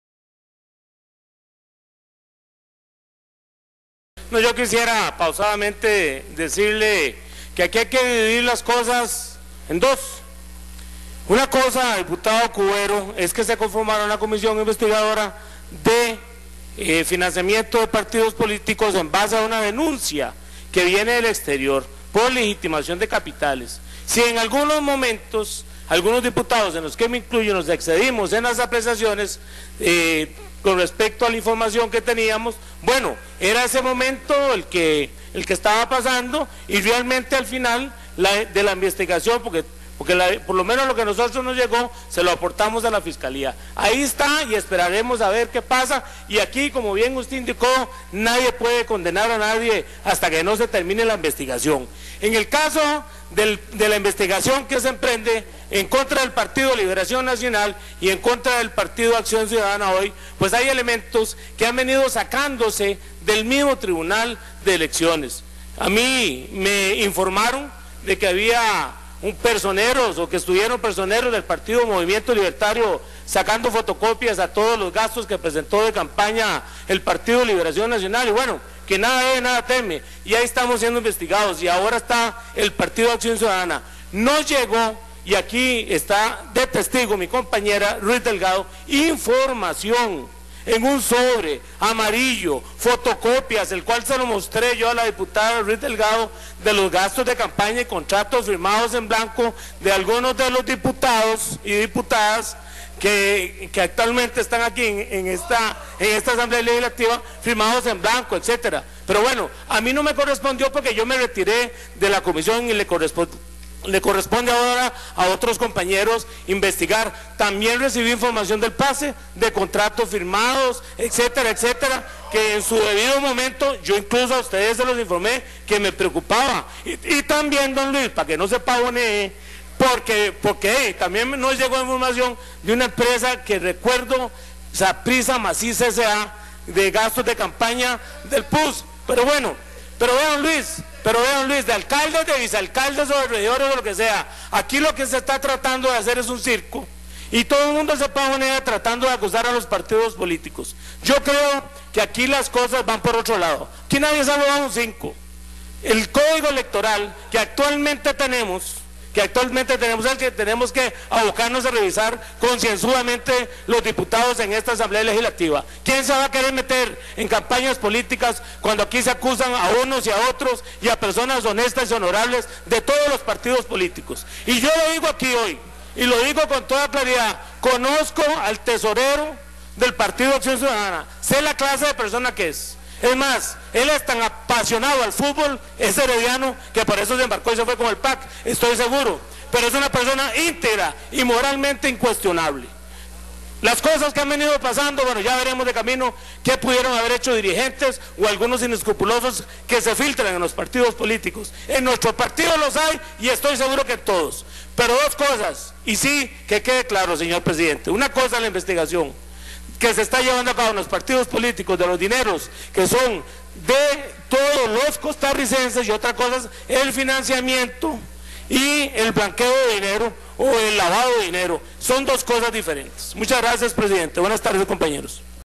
LA VOZ LIBERACIONISTA: "Nadie puede condenar a nadie hasta que no se termine investigación"
DIPUTADO VICTOR HUGO VIQUEZ CHAVERRI,  "Nadie puede condenar a nadie hasta que no se termine investigación"